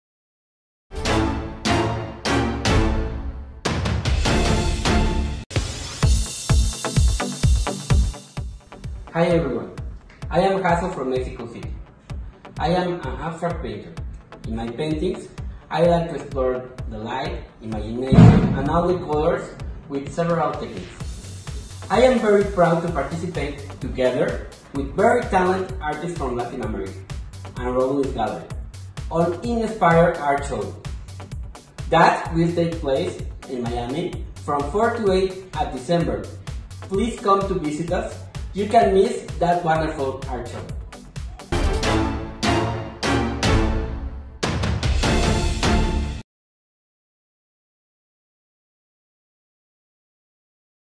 Meet JASSO (Jacinto Gonzalez Foncerrada)
Jasso is one of the independent visual artist from Latin-america that will be presenting Ralwins Art Gallery during Art Basel and Miami Art week this coming December in their event INSPIRE-MIAMI 2019. A must see event that presents a fusion of Visual Arts, Fashion and Films.

Mark your calendar and come meet Jasso as well as other amazing artists who bring us their colors, texture, concepts, culture, history, nature, strokes, stories, talents, techniques and consecration to the arts. Remember! Jasso this December 4-8 at Ampersand Studios, 31 NE 17th Street, Miami, FL 33132.